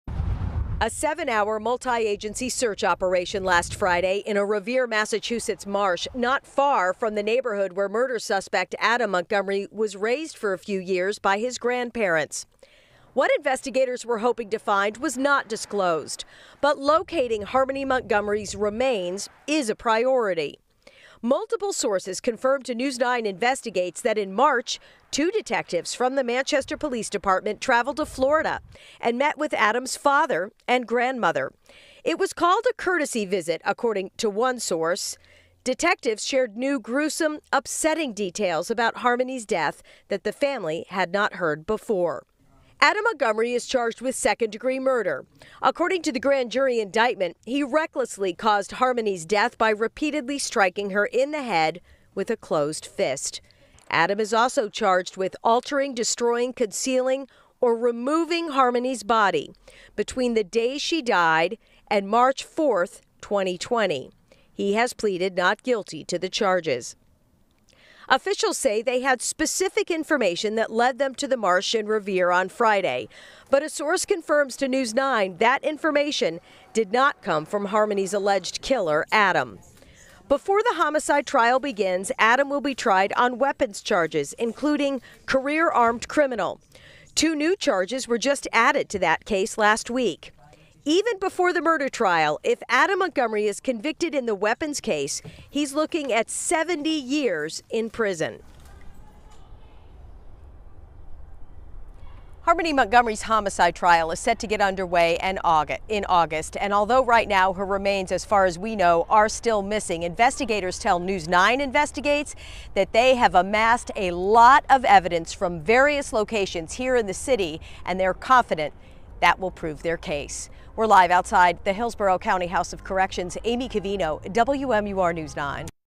Manchester detectives travel to Florida in Harmony Montgomery investigation
Manchester detectives recently traveled to Florida as they continue to investigate the death of the 5-year-old girl.